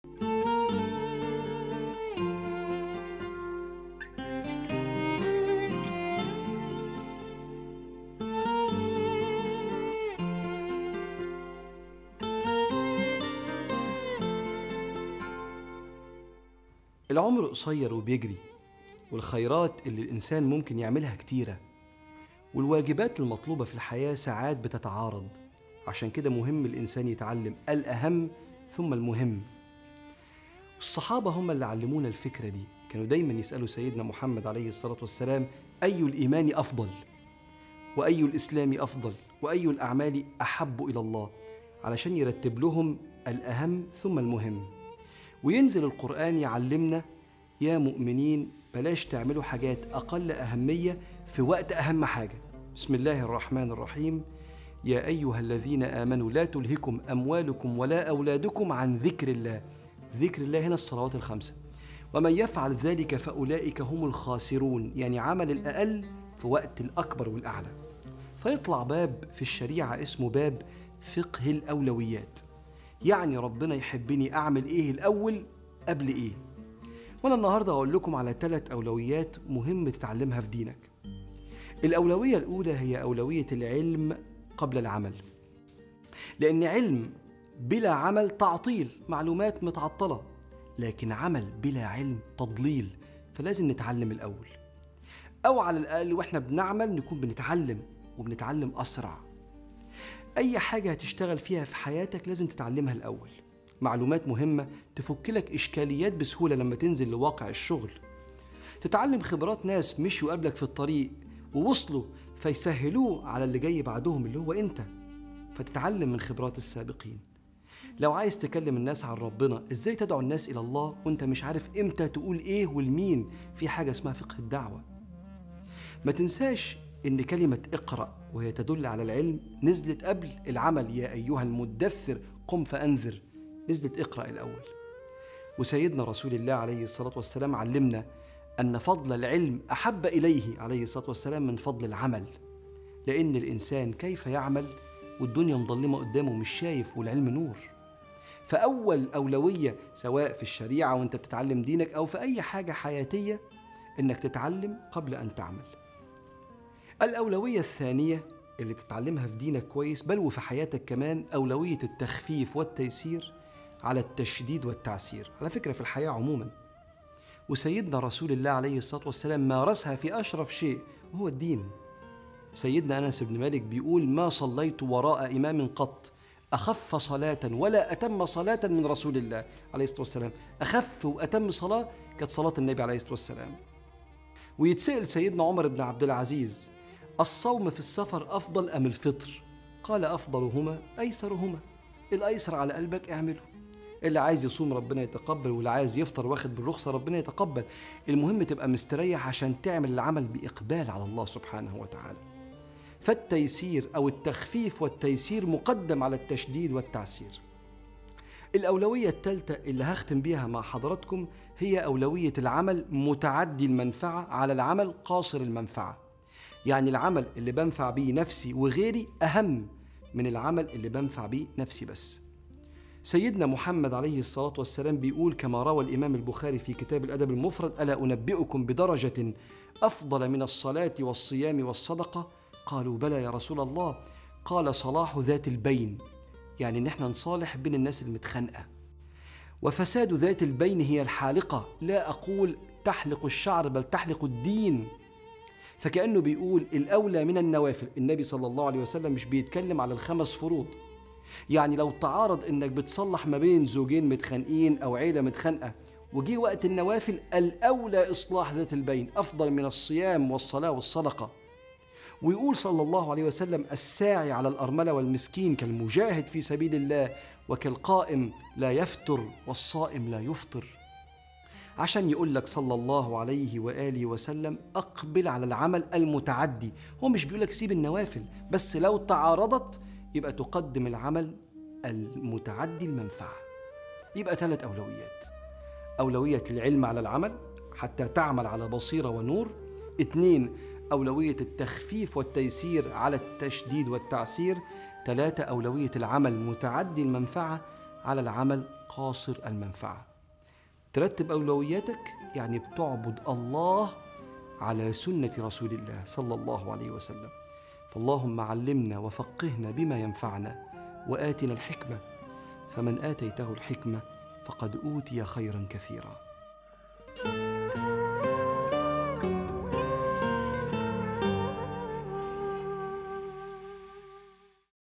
91- Priorités - "Médite 1" Mustafa Hosny
« L’âme incite au mal… Alors si elle te désobéit dans l’obéissance : Désobéis-lui –toi- dans la désobéissance »
Al Hassan Al Bassri
La vie est courte et va vite,
Et les bonnes œuvres que l’être humain peut faire sont nombreuses
Et les devoirs demandés dans la vie, parfois s’opposent…
C’est pour cela qu’il est important que l’être humain apprenne : « Le plus important », puis « L’important ».
Ce sont les compagnons qui nous ont appris cette idée,
Ils demandaient toujours à notre maître le Prophète Mohamed (bénédictions et paix soient sur lui) :
« Quelle est la meilleure Foi ? »,
Et « Quel est le meilleur Islam ? »,
Et « Quelles sont les œuvres les plus aimées par Dieu ? »,
Afin qu’il leur classe « Le plus important », puis « L’important ».
Et le Coran Descend pour nous apprendre : Ô croyants, ne faites pas des choses qui ont moins d’importance, à l’heure du plus important.
Au Nom de Dieu Le Tout Miséricordieux, Le Très Miséricordieux :
{ Ô vous qui avez cru ! Que ni vos biens ni vos enfants ne vous distraient du rappel d'Allah.}
Coran : Sourate 63 AL-MUNAFIQUN / LES HYPOCRITES en français (Verset 9)
Le rappel d’Allah = Les 5 Prières (Salates) Obligatoires.
{ Et quiconque fait cela... alors ceux-là seront les perdants. }
C'est-à-dire il a fait le moindre, à l’heure du plus grand et du plus élevé.
Donc sort une porte dans la Loi islamique (Chari’a) titrée : « La porte des priorités ».
C'est-à-dire ce que notre Seigneur Aimerait que je fasse en premier, avant autre chose.
Et aujourd’hui je vais vous citer 3 priorités, qui sont importantes à apprendre dans ta religion :
1- La première priorité est : La priorité du savoir, avant l’action.
Car un savoir, sans action = Une stérilité… Des informations stériles.
Mais une action, sans savoir = Un égarement…
Donc il faut que l’on s’instruise d’abord.
Ou au minimum, en agissant : Nous apprenons en même temps, et nous apprenons plus vite.
Chaque chose dans laquelle tu travailleras dans ta vie : Il faut que tu l’apprennes d’abord,
Des informations importantes, qui te résoudront facilement des problématiques, lorsque tu seras sur le terrain du travail.
Tu apprends des expériences de personnes que tu croises sur le chemin, et qui sont arrivées à destination,
Du coup elles facilitent pour ceux qui arrivent après eux, c'est-à-dire toi.
Tu apprends donc des expériences de ceux qui ont précédé.
Si tu souhaites parler aux gens de notre Seigneur :
Comment pourrais-tu appeler les gens vers Dieu, si tu ne sais pas quand, quoi dire, et à qui…
Il existe une chose appelée « La science de l’Appel vers Dieu (Da’wa) ».
N’oublie pas que le verbe { Lis }
Coran : Sourate AL-ALAQ 96 / L'ADHÉRENCE en français (Verset 1)
Est descendu avant l’action :
{ 1 Ô, toi (Muhammad) ! Le revêtu d'un manteau !
2- Lève-toi et avertis. }
Coran : Sourate 74 AL-MUDDATTIR / LE REVETU D'UN MANTEAU en français
Le verbe { Lis } est Descendu en premier.
Et notre maître le Messager de Dieu (bpsl), nous a appris que la faveur du savoir, est plus aimée par lui, que la faveur de l’action.
Car comment pourrait agir l’être humain, si le monde est sombre devant lui, et qu’il ne voit pas ?
Et le savoir est une lumière.
Donc la 1ère priorité, soit dans la Loi islamique (Chari’a), en apprenant ta religion, ou dans n’importe quelle chose de la vie : C’est de t’instruire, avant d’agir.
2- La deuxième priorité que tu apprends bien dans ta religion, et même dans ta vie : La priorité de l’allégement et de la facilitation, avant la surcharge et la complication.
C’est une idée pour la vie en général,
Et notre maître le Messager de Dieu (bpsl) l’a appliquée dans la chose la plus honorable = Qui est La religion.
Notre maître Anass Ibn Malek dit :
« Je n’ai jamais Prié derrière un imam, ayant une Prière (Salate) légère et parfaite : Comme celle du Messager de Dieu (bpsl) ».
La Prière la plus légère et la plus parfaite, était celle du Prophète (bpsl).
Et on a posé la question à notre maître Omar Ibn AbdelAziz :
« Est-il préférable de Jeûner pendant le voyage, ou de manger ? »,
Il a répondu :
« Le plus préférable des deux, est le plus facile »,
Ce qui est le plus facile pour ton cœur : Fais-le.
Celui qui souhaite Jeûner : Que Dieu l’Accepte.
Et celui qui souhaite manger : Il a une autorisation, que Dieu l’Accepte.
L’important c’est d’être à l’aise, afin d’accomplir l’œuvre avec enthousiasme avec Dieu (Exalté Soit-Il).
Donc la facilitation,
Ou bien l’allégement et la facilitation, sont prioritaires sur la surcharge et la complication.
3- La troisième priorité par laquelle je terminerai avec vous : C’est la priorité des œuvres multi-utiles, sur les œuvres utiles.
C'est-à-dire l’œuvre par laquelle je suis utile à moi-même et aux autres : Est plus importante que l’œuvre par laquelle je suis utile à moi-même uniquement.
Notre maître Mohamed (bpsl) dit, comme l’a rapporté l’imam Al Boukhari dans le livre « La politesse singulière » :
« Ne voudriez-vous pas que je vous indique un degré meilleur que la Prière (Salate), le Jeûne, et l’aumône